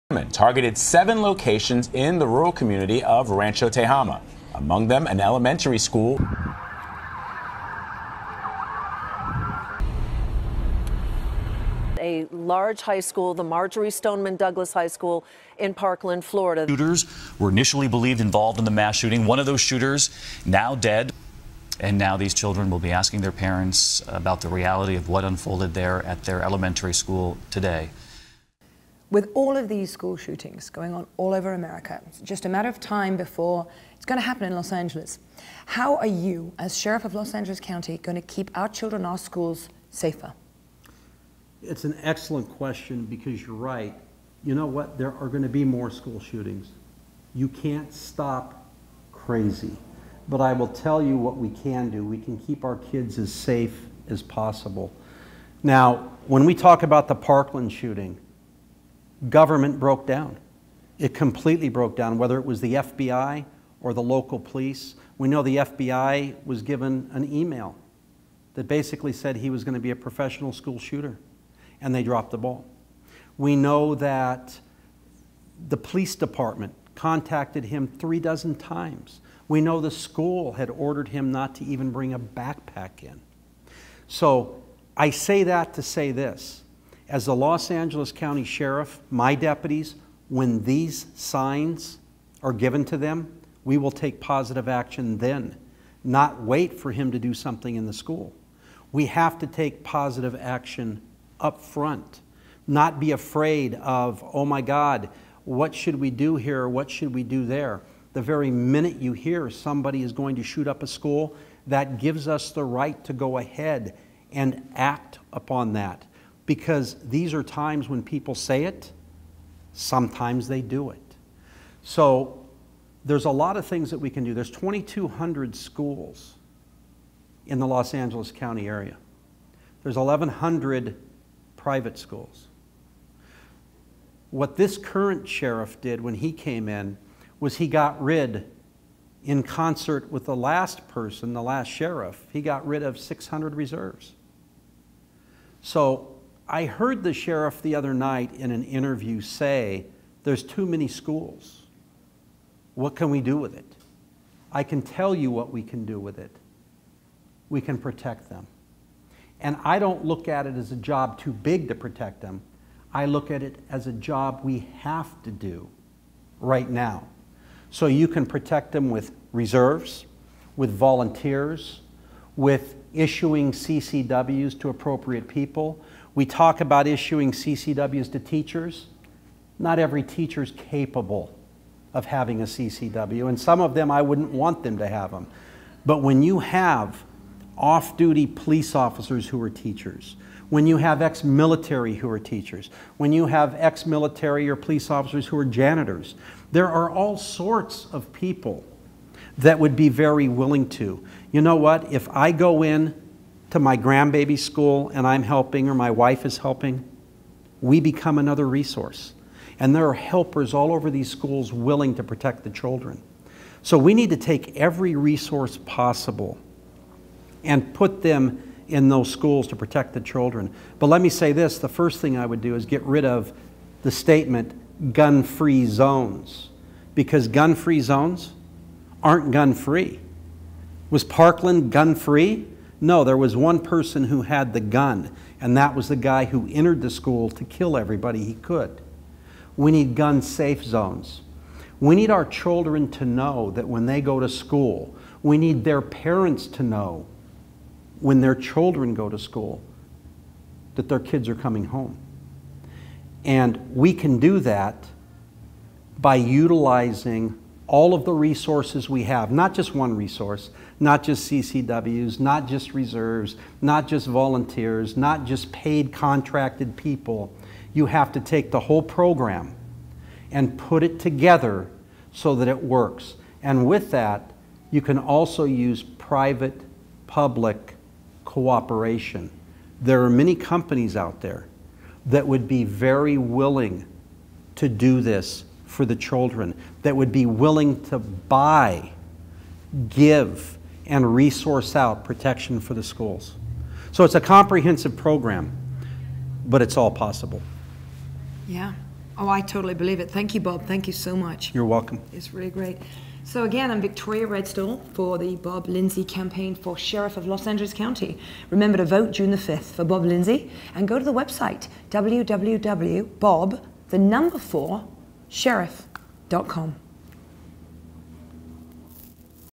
Protecting Los Angeles County's 2200 schools is not an option.
Bob Lindsey for Los Angeles County Sheriff. Bob Lindsey has strong views about protecting the 2200 schools and the children within this vast system. There is not a single approach, rather it will take a blending and cooperative system that will involve all aspects and resources available. Vote for Bob Lindsey on June 5th, 2018, "Let's Take The Politics Out of Law Enforcement"!